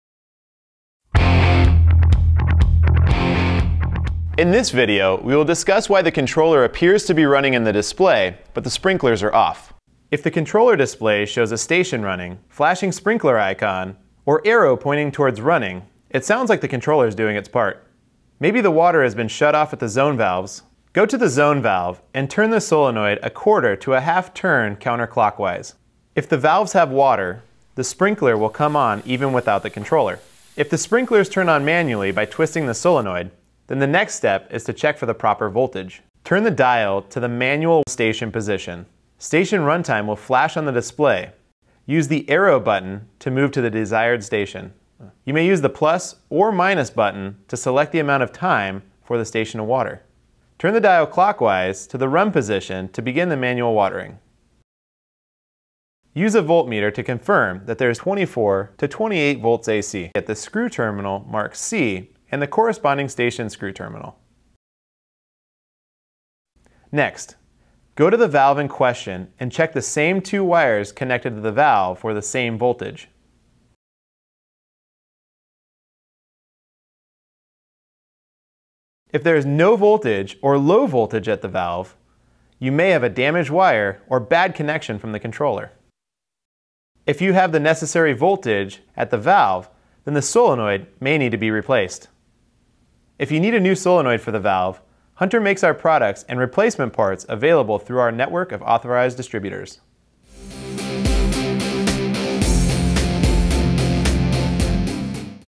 Pro-C® No Zones Running
Pro-C - Flashing Sprinkler Icon, No Zones Running

If the controller display shows a station running, flashing sprinkler icon,  or arrow pointing towards running, it sounds like the controller is doing its part. Maybe the water has been shut off to the zone valves. Go to the zone valve and turn the solenoid (where the wires are connected) 1/2 turn counter clockwise. If the valves have water, the sprinklers will come on even without the controller. See illustration below for activating the valve solenoid manually:

If the sprinklers turn on manually by twisting the solenoid, then the next step is to check for the proper voltage.

To confirm the proper voltage is getting to the valves using a voltmeter, follow the steps below:

If you need a new solenoid for the valve, Hunter makes our products and replacement parts available through our network of authorized distributors.

Please click on the link below to find an Authorized Hunter Distributor in your area Distributor Locator
Step 1

Turn dial to MANUAL-ONE STATION position. Station run time will flash in the display. Use the arrow button to move to the desired station. You may use the plus (+) or minus (-)button to select the amount of time for a station to water.  Turn the dial clockwise to the RUN position to run the station.
Step 2

Use a volt meter to confirm there is 25-28 volts VAC at the screw terminals marked "C" and the corresponding station screw terminal ("C" and "2" for example).
Step 3

Next, go to the valve in question and check the same two wires connected to the valve for the same voltage.
Step 4

If there is no voltage or low voltage at the valve, you may have a damaged wire or bad connection from the controller.
Step 5

If you have necessary voltage (25-28 VAC) at the valve then the solenoid may need to be replaced.